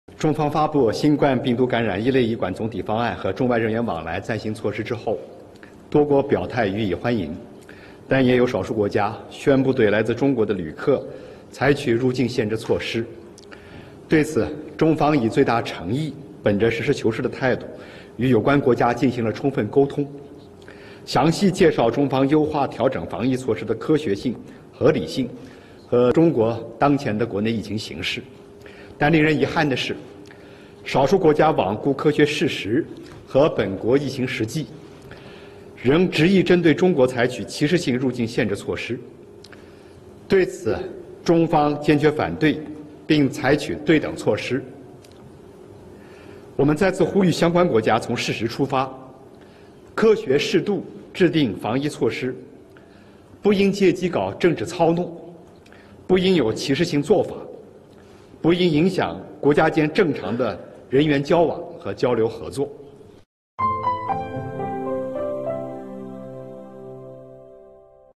中国暂停向日本游客发放签证？外交部回应
1月10日，外交部发言人汪文斌主持例行记者会。有记者提问：据报道，中国发布了暂停向日本游客发放签证的通知，发言人能否证实这一消息？有无更多信息可提供？
        汪文斌表示，中方发布新冠病毒感染“乙类乙管”总体方案和中外人员往来暂行措施之后，多国表态予以欢迎。但也有少数国家宣布对来自中国的旅客采取入境限制措施，对此中方以最大诚意，本着实事求是的态度，与有关国家进行了充分沟通，详细介绍中方优化调整防疫措施的科学性、合理性和中国当前的国内疫情形势。但令人遗憾的是少数国家罔顾科学事实和本国疫情实际，仍执意针对中国采取歧视性入境限制措施。对此中方坚决反对并采取对等措施。